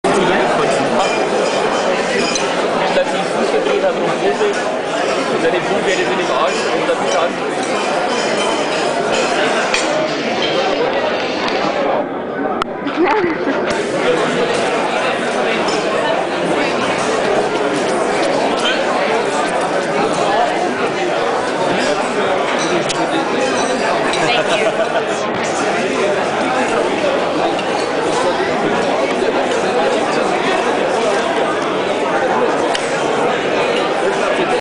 Eating at Augustiner-Bräustuben
True Bavarian food with good friends, Hans-Peter, Florian, Jochen and Thomas!!